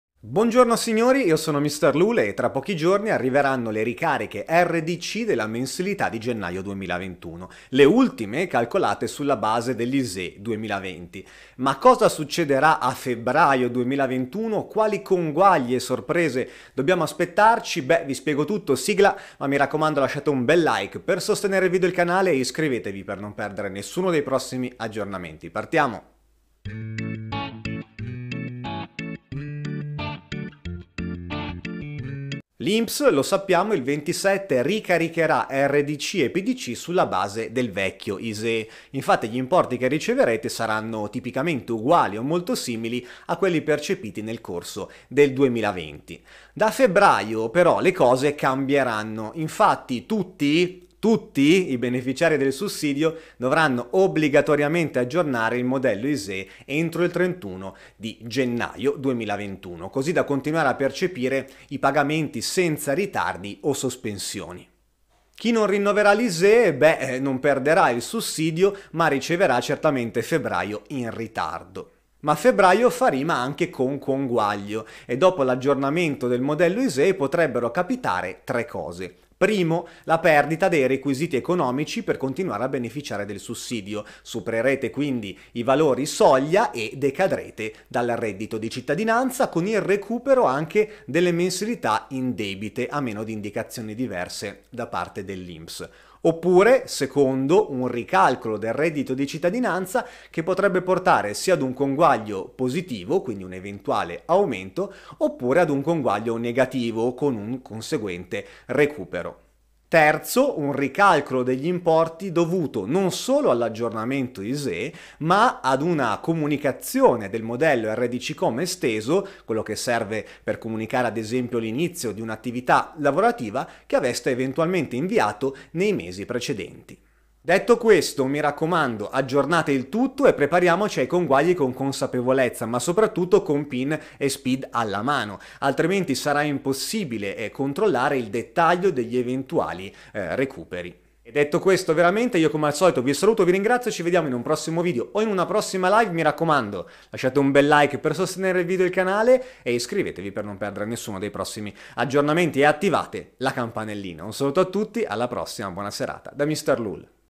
REDDITO DI CITTADINANZA: AUMENTO CONGUAGLIO RECUPERI A FEBBRAIO 2021 [3 COSE DA SAPERE]
Tra pochi giorni arriveranno le ricariche rdc della mensilità di gennaio 2021, le ultime calcolate sulla base dell’isee 2020.
Ma cosa succederà a Febbraio 2021? quali conguagli e sorprese dobbiamo aspettarci?vi spiego tutto sigla ma

L’Inps lo sappiamo il 27 ricaricherà rdc e pdc sulla base del vecchio ISEE, infatti gli importi che riceverete saranno tipicamente uguali o molto simili a quelli percepiti nel corso del 2020.

Da febbraio le cose però cambieranno
infatti Tutti - tutti - i beneficiari del sussidio dovranno obbligatoriamente aggiornare il modello Isee entro il 31 Gennaio 2021 così da continuare a percepire i pagamenti senza ritardi o sospensioni.
Chi non rinnoverà l’isee non perderà il sussidio ma riceverà febbraio in ritardo.

ma Febbraio fa rima con conguaglio
e dopo l’aggiornamento del modello Isee potrebbero capitare tre cose: